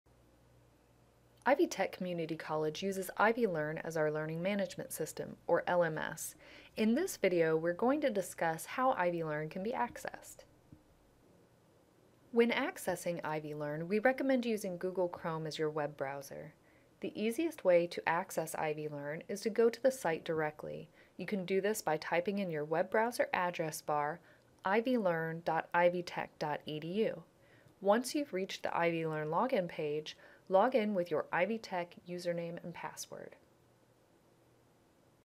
Accessing IvyLearn
This video demonstrates how to access IvyLearn.

Access IvyLearn by going directly to ivylearn.ivytech.edu and logging in with your Ivy Tech username and password.